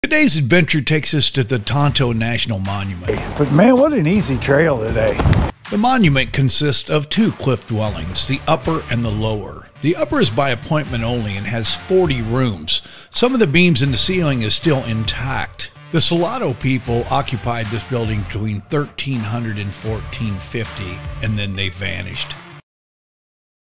HIKING TO 800 YEAR OLD CLIFF DWELLINGS
Upper Cliff Dwelling tours are fully booked through the April 2022 season. Cancellations may occur, but they are rare. Reservations for next season will be taken starting October 1, 2022 for November 2022-April 2023.

The Upper Cliff Dwelling is accessible only by guided tour with a reservation. Tours are offered November thru April, every Friday, Saturday, Sunday, and Monday starting at 10:00 am.

Arizona contains some of the nation's, and indeed the world's, greatest archeological sites. Nestled in a cave overlooking Tonto Basin is the 40-room Upper Cliff Dwelling. Many theories have been presented as to why people began building here. Protection from the elements is a possibility. The cave is dry even during the worst weather and receives morning sun in winter and cooling shade in summer. Or perhaps people were protecting themselves from their neighbors or were trying to get away from crowded conditions on the basin floor. A construction sequence of the Upper Cliff Dwelling gives details on how and in what order the rooms were built.

Construction of the Upper and Lower Cliff Dwellings began about 1300 CE and continued until the Basin was abandoned, between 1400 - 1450 CE. The size of the cave, 70 feet wide, 80 feet high, and 60 feet deep, allowed for living quarters, many with second story rooms. A large open work area contained a cistern, capable of holding approximately 100 gallons of water.

When you visit the Upper Cliff Dwelling, note the partially intact roofs, stone and slab door lentils, portholes, second story rooms, and third story parapet walls which served as balconies for outside living areas. Please take a few minutes to familiarize yourself with this site etiquette guide that will facilitate an enjoyable visit for you.

Small doorways helped control room temperatures. The Salado sealed off some doors and made others smaller over time. Large doorways would have allowed for construction beams to pass through, and may have been filled in after construction was done. Some doorways are shaped like a "T" or half-"T". These may have reduced drafts or helped someone balance while entering the room. It is still possible to see fingerprints of the ancient people in the masonry of these remarkable structures.Instagram
Camera Bag: